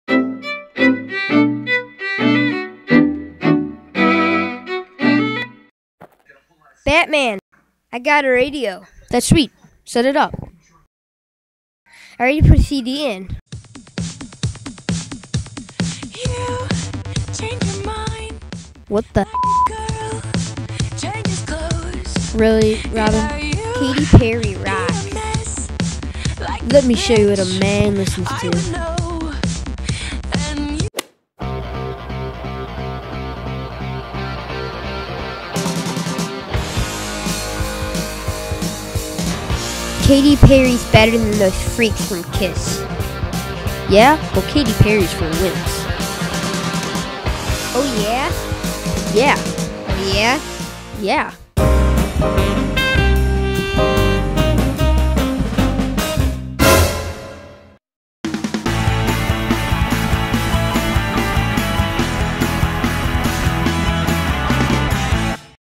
Respect Music Choice (Batman)
Batman and Robin can't respect eachothers' music choices